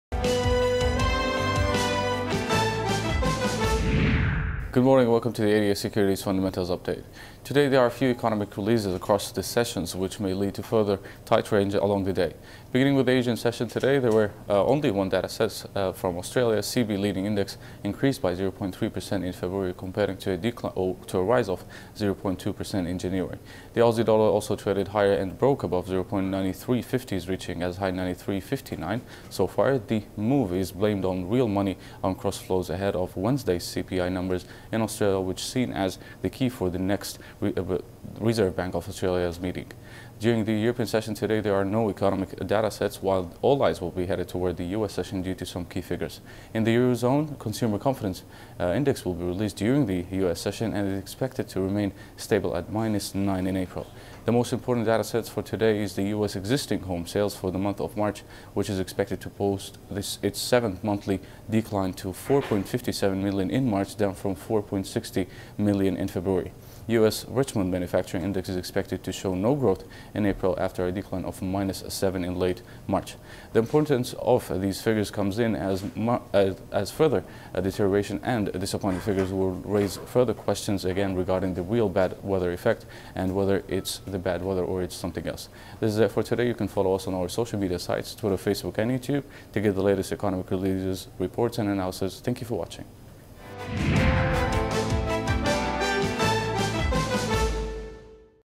Daily Fundamental Update - 22/04/2014
Today we take a look at the important figure of CB Leading Index released from Australia and its impact on AUDUSD; no releases from Europe but we preview some key numbers expected from the US later in the day.